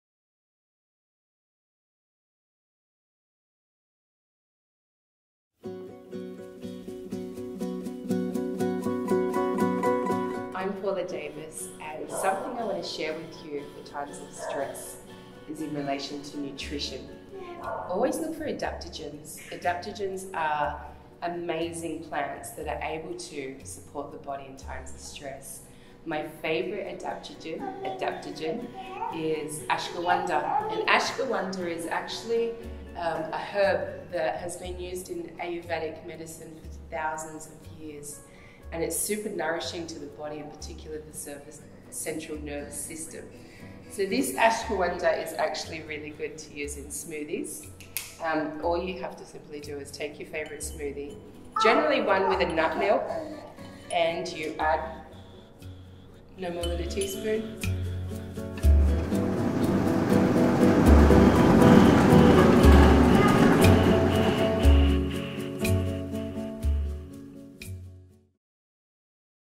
BODY / Ashwaganda Smoothie with Paula Davis
Proper nutrition is essential to support the body in times of stress — @pauladavis_natural_living shares a simple recipe to support the body in times of stress using natural herbs and adaptogens. Show us your recipes with us